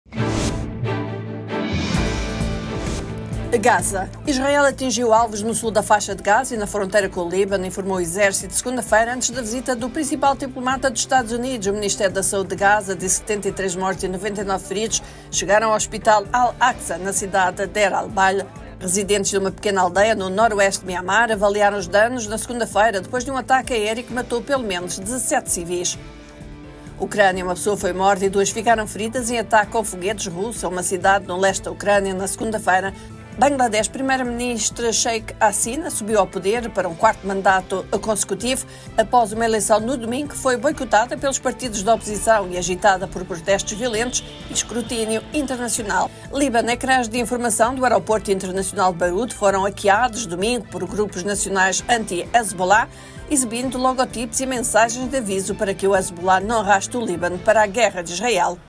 Manchetes mundo: Myanmar - Ataque aéreo que mata pelo menos 17 civis
Israel atingiu alvos no sul da Faixa de Gaza e na fronteira com o Líbano.
Líbano: Os ecrãs de informação do aeroporto internacional de Beirute foram hackeados no domingo por grupos nacionais anti-Hezbollah.